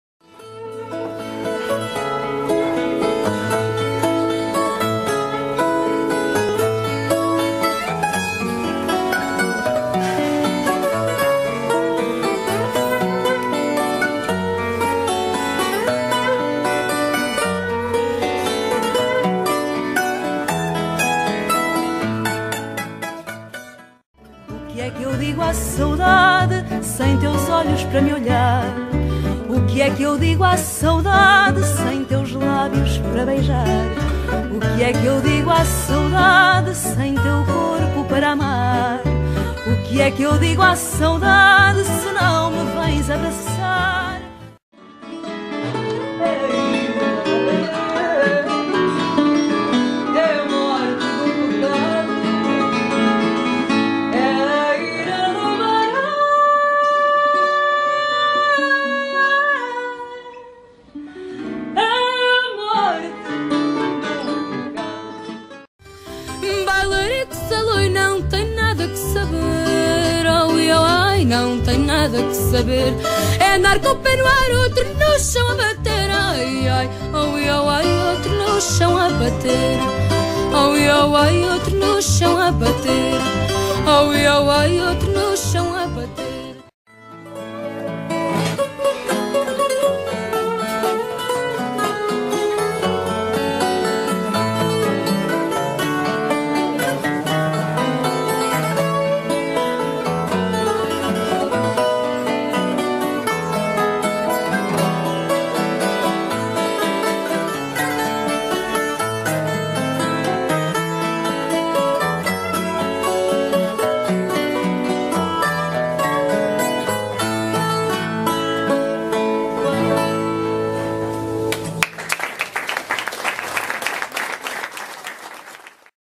Fado no Feminino Samouco Maio 2013
Vídeo de Apresentação de "Fado no Feminino" que se realizará na Quinta de São Braz, no Samouco em 4 de Maio de 2013, com a presença das fadistas Joana Amendoeira, Vicenta, Teresinha Landeiro e Fernanda Vila Cova, acompanhadas por Pedro Amendoeira e Nélson Aleixo, para além de participações especiais do Grupo Coral do Samouco e do Quinteto de Metais da Banda do Samouco, numa produção de ANS Eventos Musicais.